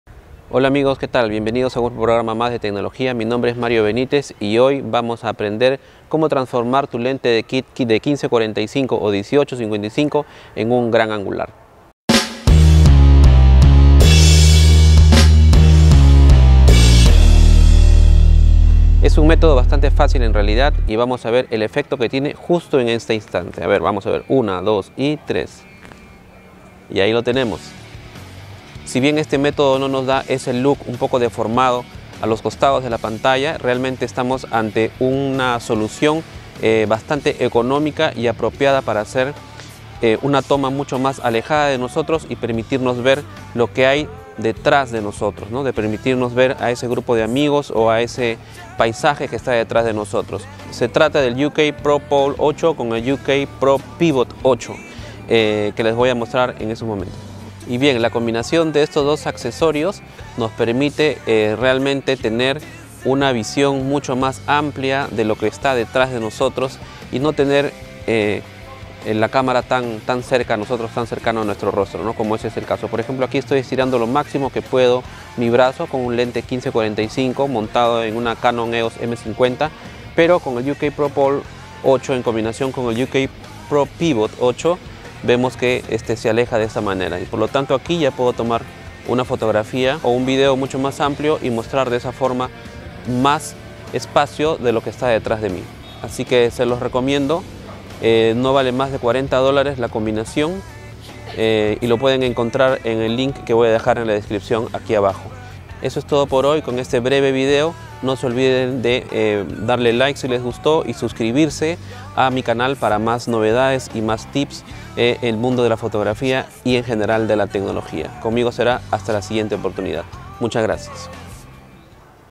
Una solución práctica y económica para que tu lente de kit sea como un angular
Esta solución económica y práctica te permite transformar tu lente de kit en uno gran angular. Se trata del UK Pro Pole 8 + UK Pro Pivot 8.